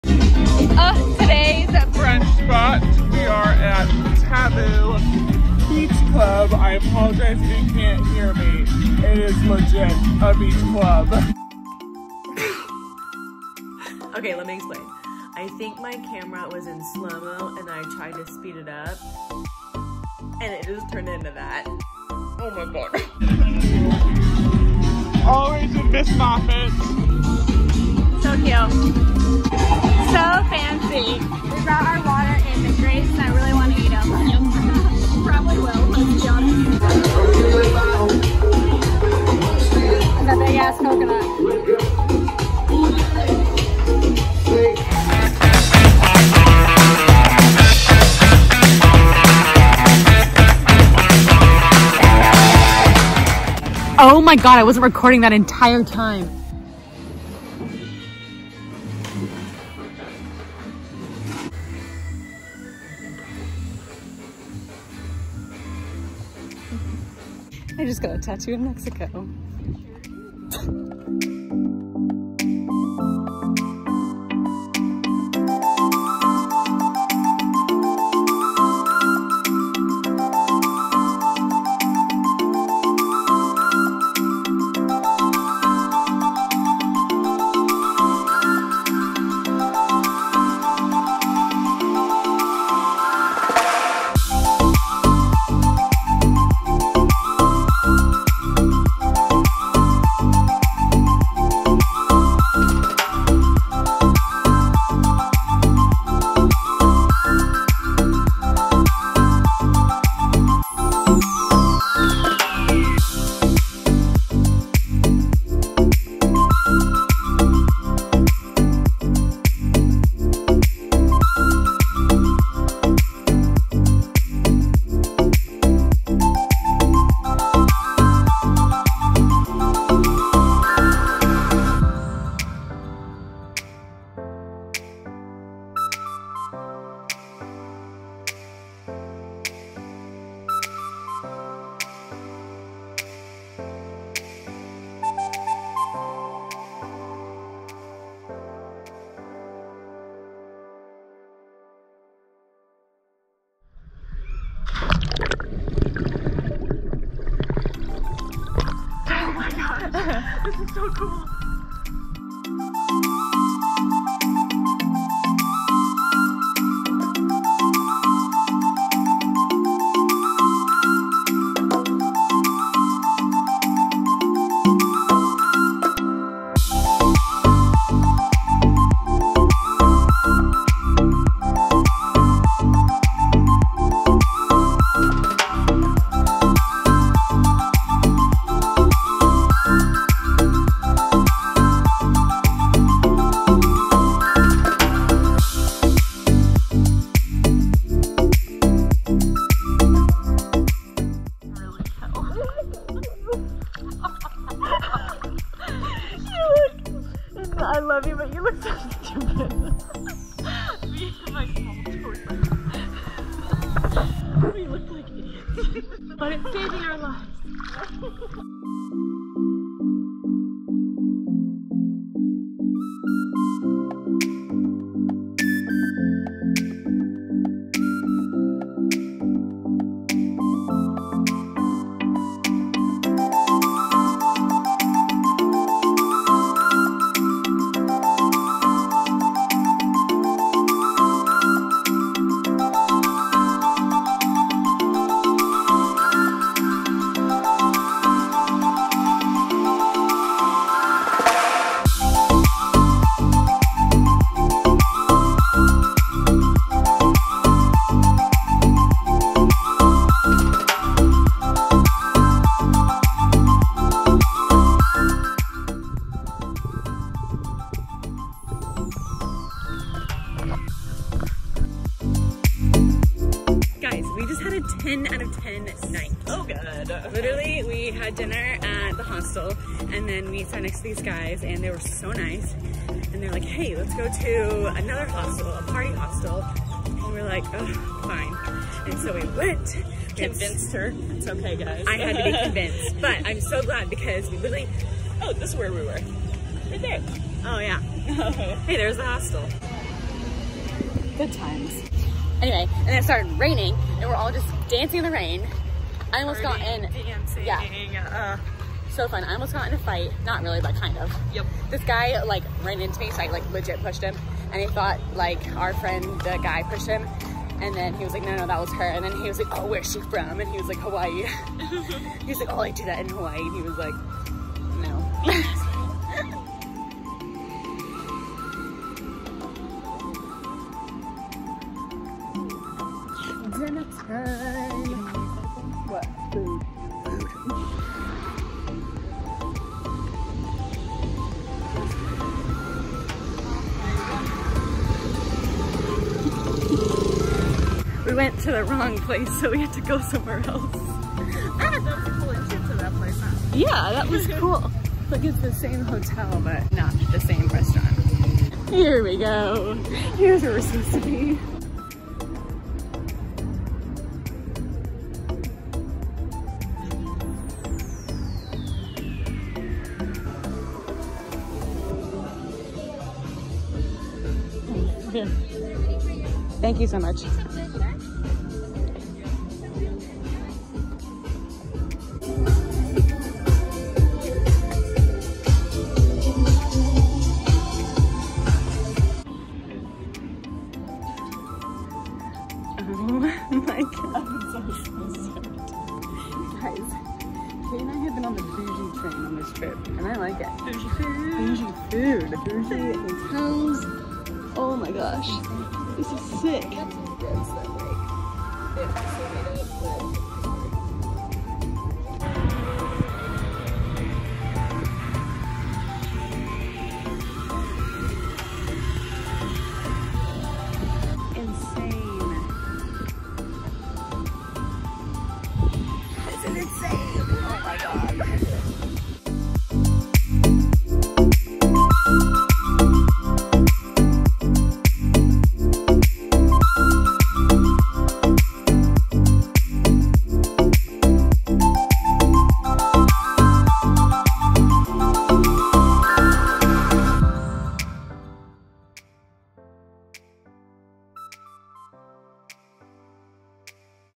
Tulum Part 2 | MEXICO TRIP VLOG #6
Aloha y'all!! We're in MEXICO!! So many vlogs to come:) follow along as Katie and I take Mexico🇲🇽🇲🇽

↠ GIGIPIP HATS: Use code JILWHITE